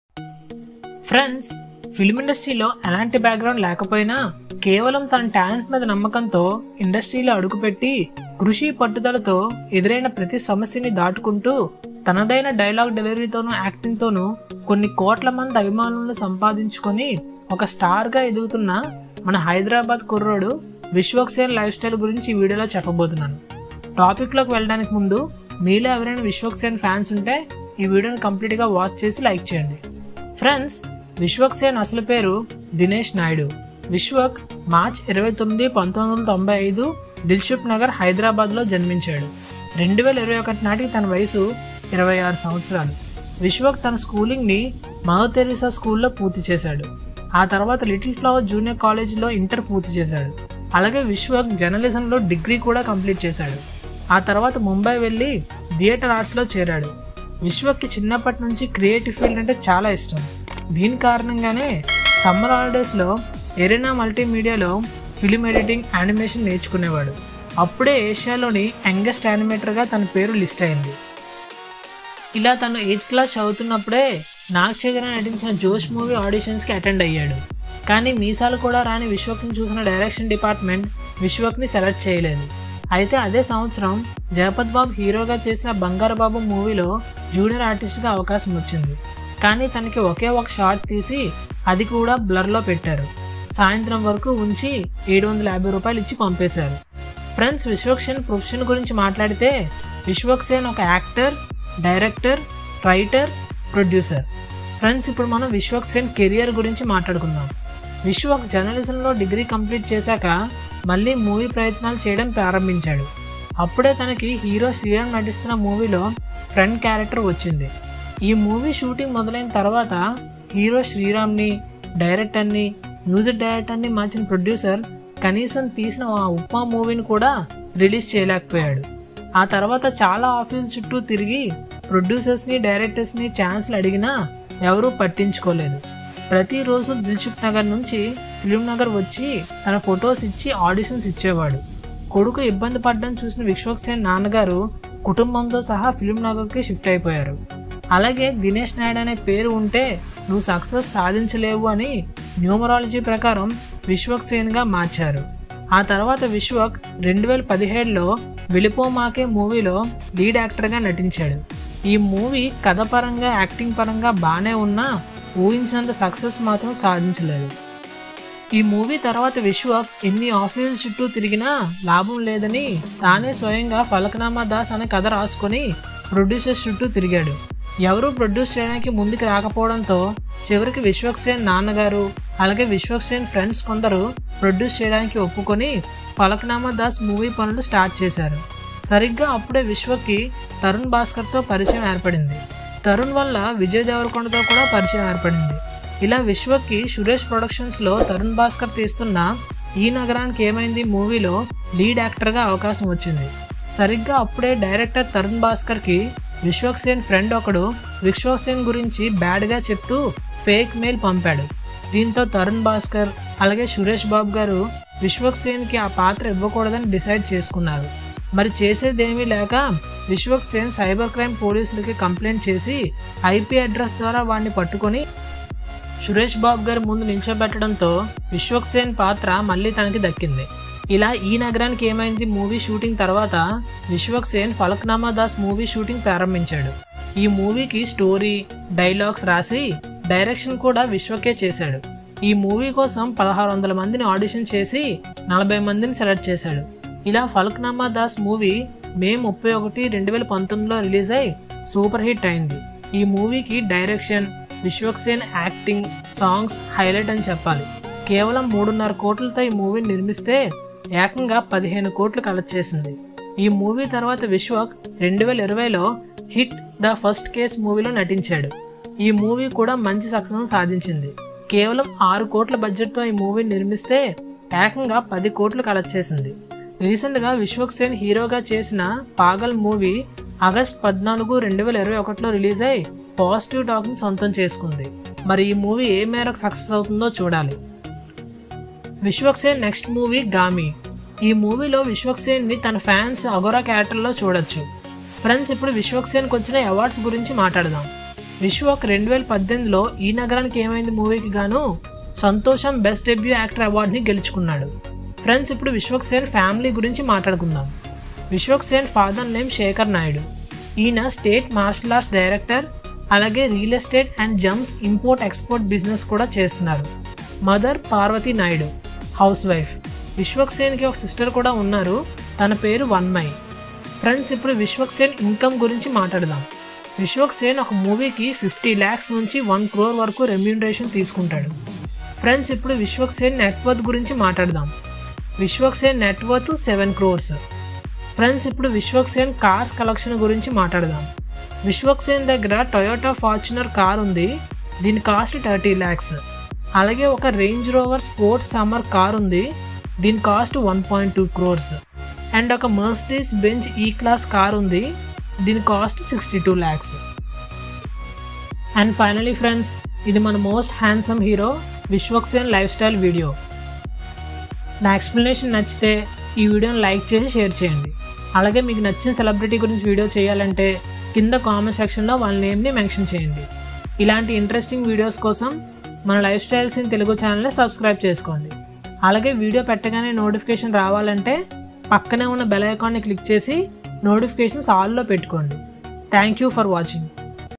Vishwak Sen Lifestyle 2021, Income, House, Cars, Family, Biography, Movies, & Net Worth in Telugu
This video is in our own language Telugu.